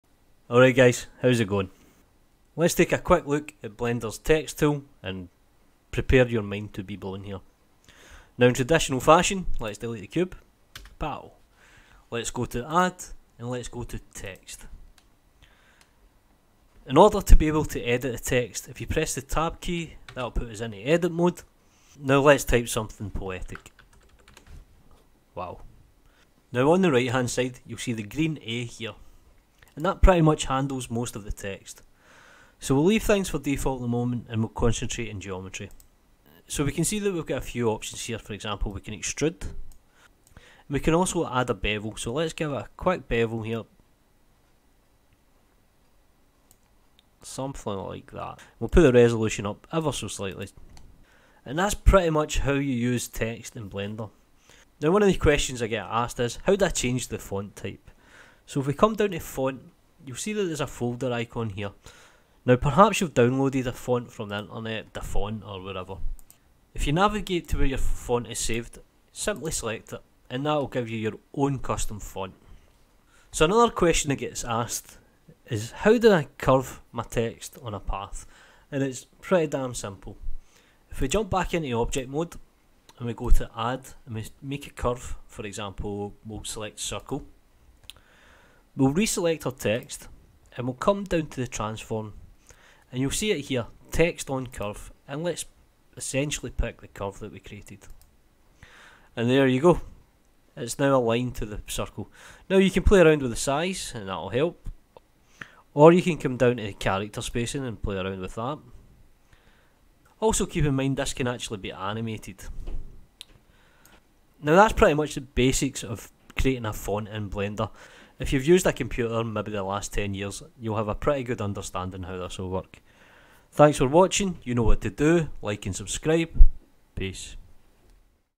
Basic: Blender 2.8 Tutorial Text Tool and Text on Curve
Create beautiful custom beveled text inside Blender 2.8 and align to curve